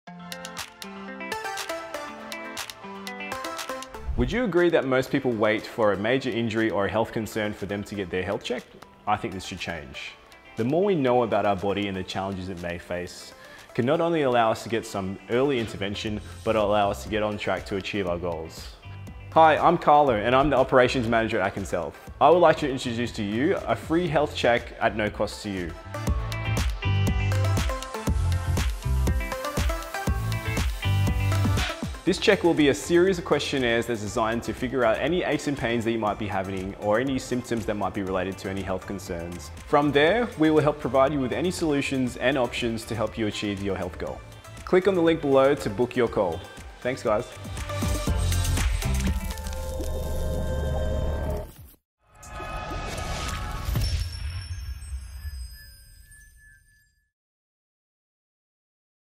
Complimentary Health Check
Join us for an Injuries, Movement, and Pain management health check with our team to become aware of your body's signs about your health!